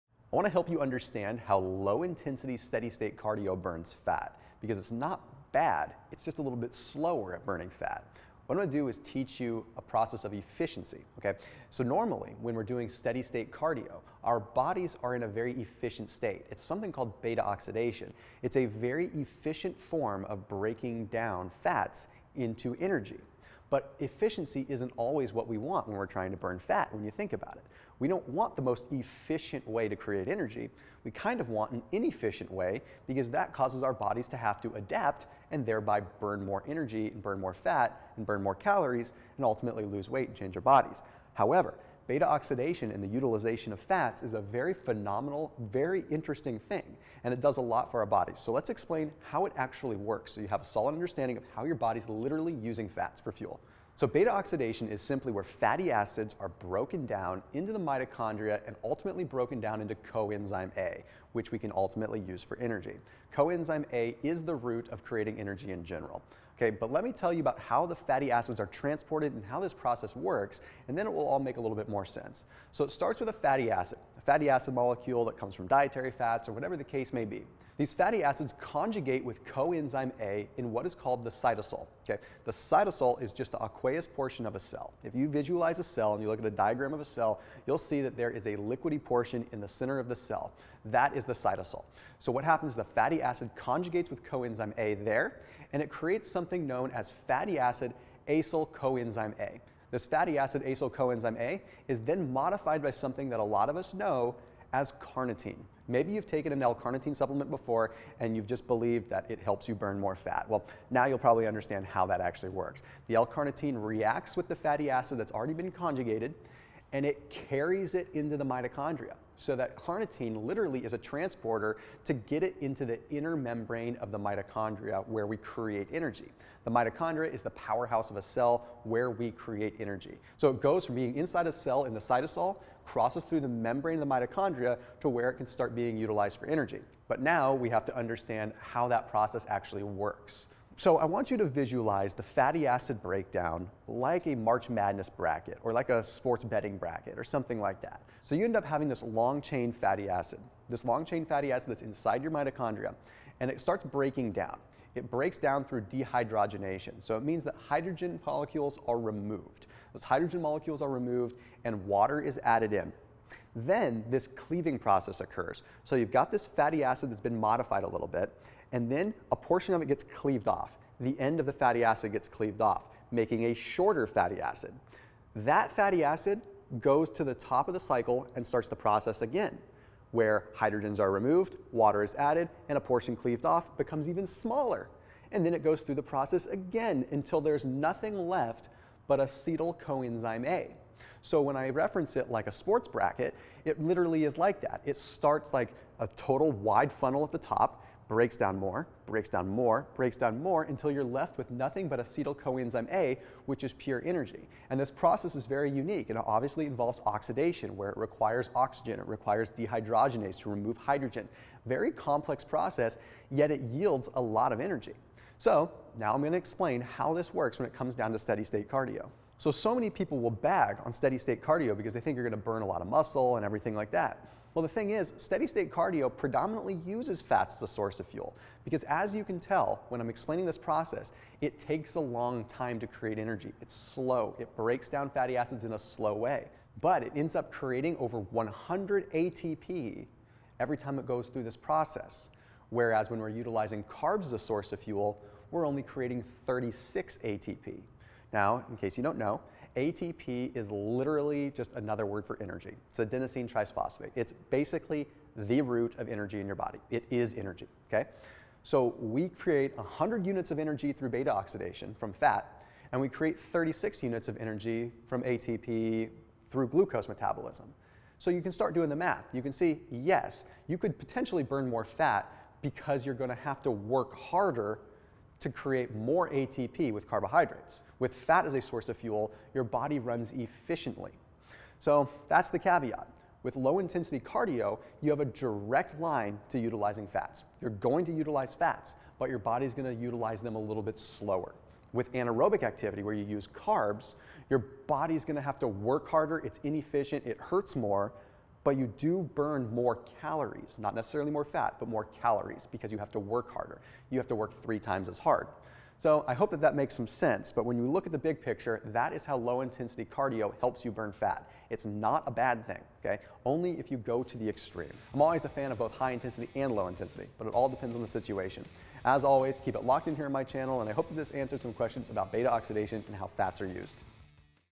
The Effect of Low Intensity Steady State Cardio
The Effect of Low Intensity Steady State Cardio - Thomas DeLauer

beta-oxidation is the catabolic process by which fatty acid molecules are broken down in the cytosol in prokaryotes and in the mitochondria in eukaryotes to generate acetyl-CoA

During fatty acid beta oxidation long chain acyl-CoA molecules - the main components of FA’s - are broken to acetyl-CoA molecules

How fatty acids are broken down and used to generate ATP

Fatty acids provide highly efficient energy storage, delivering more energy per gram than carbs

In tissues with high energy requirement, such as heart, up to 50–70% of energy, in the form of ATP production, comes from fatty acid (FA) beta-oxidation

During fatty acid β-oxidation long chain acyl-CoA molecules – the main components of FAs – are broken to acetyl-CoA molecules

Fatty acid transport into mitochondria

Fatty acids are activated for degradation by conjugation with coenzyme A (CoA) in the cytosol

The long-chain fatty-acyl-CoA is then modified by carnitine palmitoyltransferase 1 (CPT1) to acylcarnitine and transported across the inner mitochondrial membrane by carnitine translocase (CAT)

CPT2 then converts the long chain acylcarnitine back to long-chain acyl-CoA before beta-oxidation (1,2)

Low Intensity

LISS is a low-intensity cardio workout that calls for typically 30 to 60 minutes - roughly 60% of maximal heart-rate effort

The aerobic system kicks in when the muscles cannot create enough energy without the aid of oxygen - although this system uses blood glucose and glycogen, it can also burn fat as well, yielding us the most ATP out of any macronutrient

The only problem is that the aerobic system is the slowest of all energy systems and may not predominantly burn fat at first

The oxidation of a single fatty acid molecule may yield over a hundred molecules of ATP

The oxidation of a single molecule of glucose, or blood sugar, yields only about 36 molecules of ATP  (1,2)

References

1) Fat Oxidation During Exercise. (2012, November 2).

2) Fatty acid beta oxidation | Abcam. (2018, March 5).